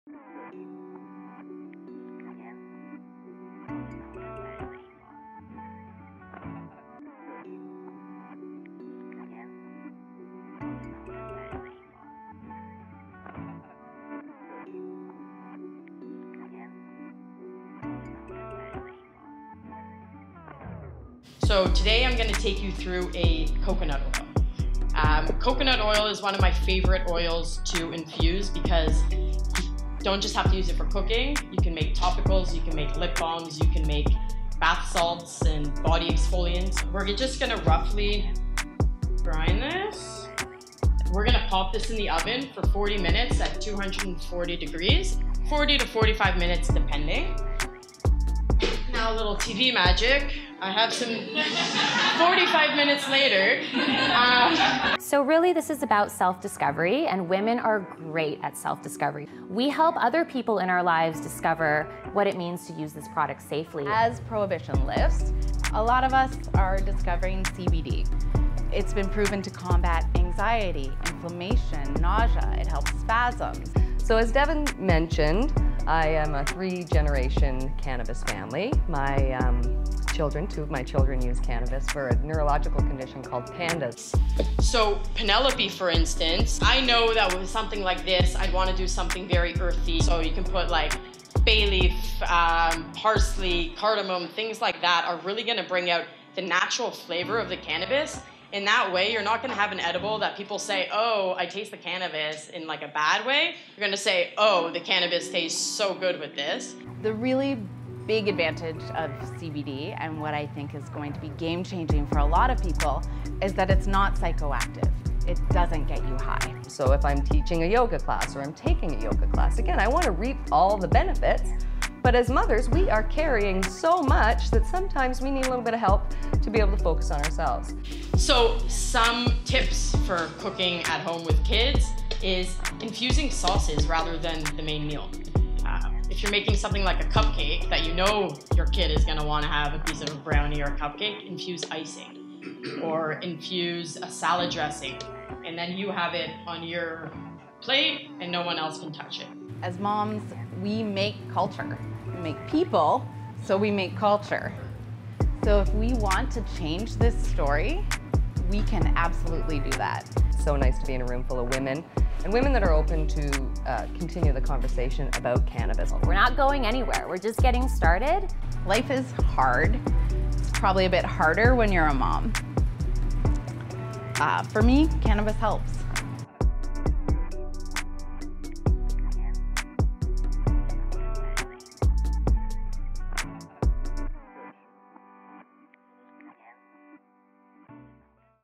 Hempster Presents: Motherhood, There's a Strain for That
On Thursday, May 3, more than 30 journalists, influencers and moms gathered at The Edible Story in downtown Toronto for Hempster’s inaugural Mother’s Day event.